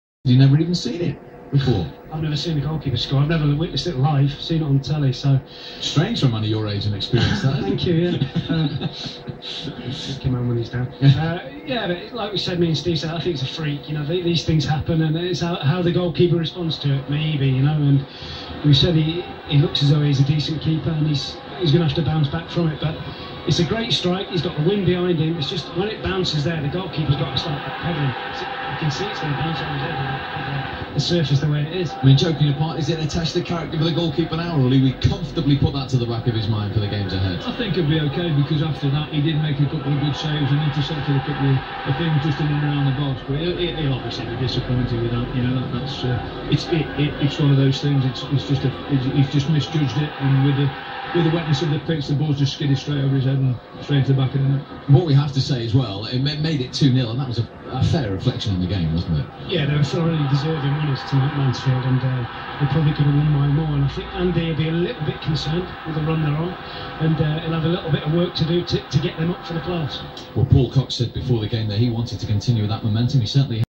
Mansfield Town Keeper Alan Marriott scores against Wrexham - 20/04/12
Mansfield Town Goalkeeper Alan Marriott scores direct from a drop kick against Wrexham to put the Stags 2-0 in a Conference League match, incrediable goal even if the weather aided it a bit!